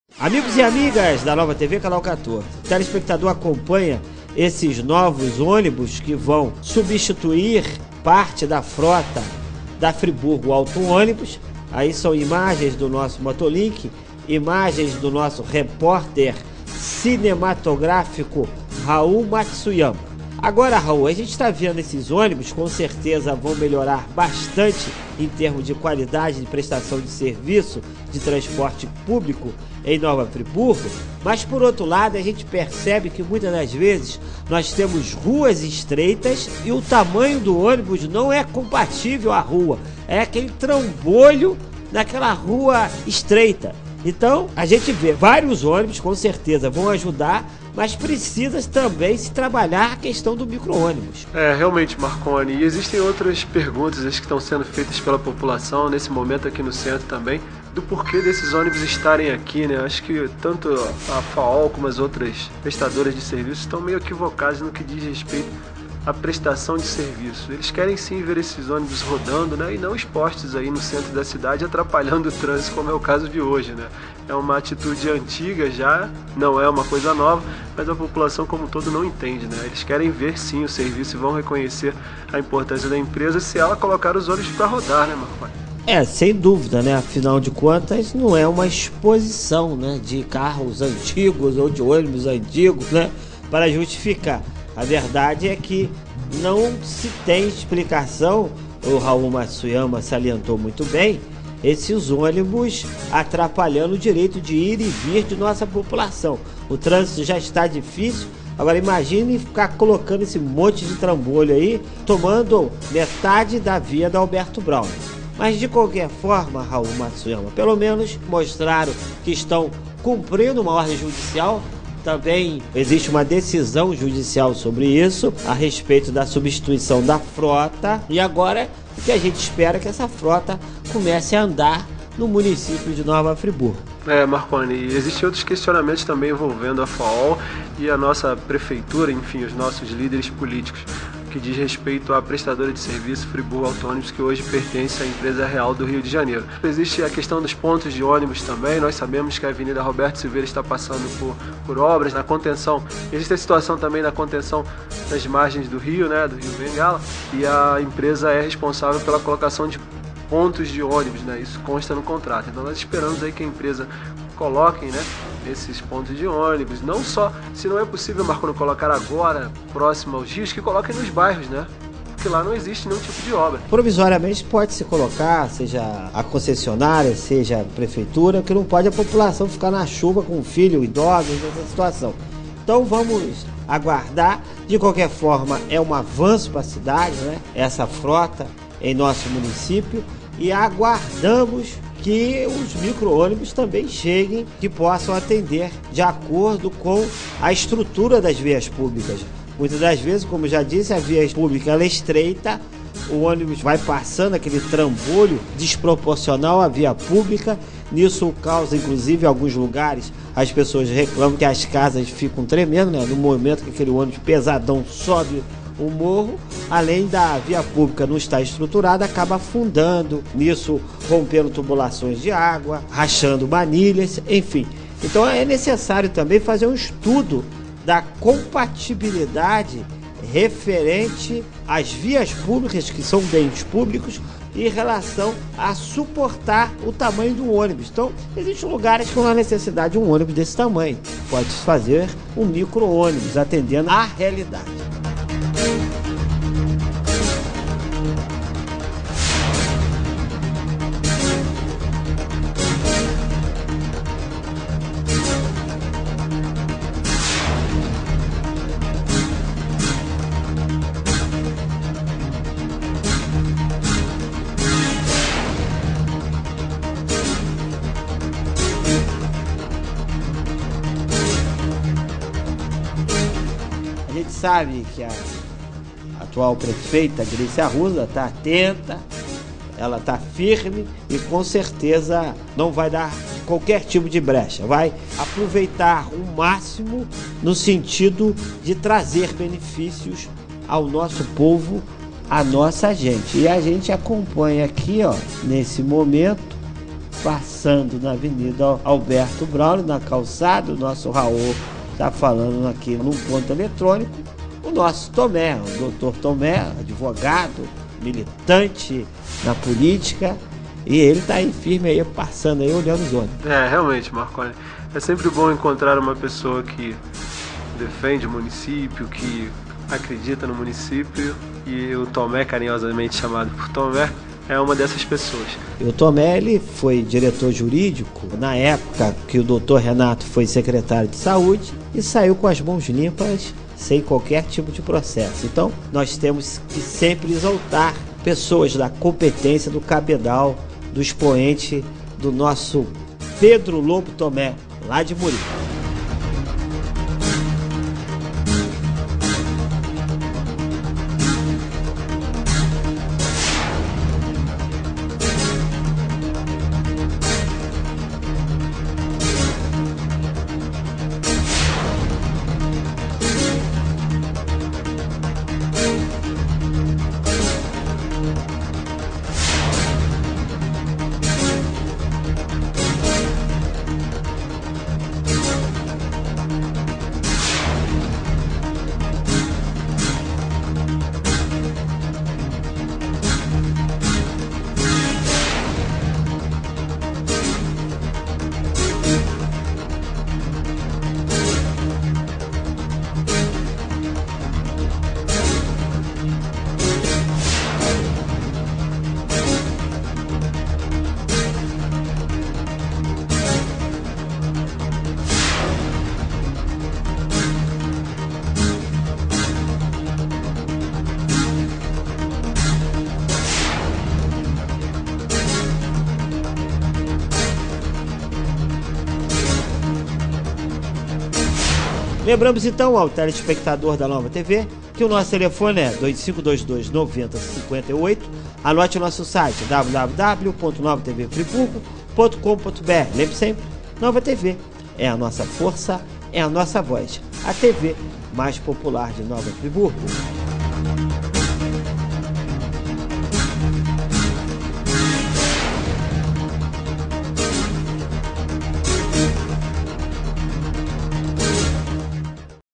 PLANTÃO NOVA TV FRIBURGO RJ FAOL RECEBE NOVA FROTA - CAOS NO TRÂNSITO 08 03 14
PLANTÃO NOVA TV FRIBURGO RJ FAOL RECEBE NOVA FROTA - MOTORISTAS RECLAMAM DO CAOS NO TRÂNSITO 08 03 14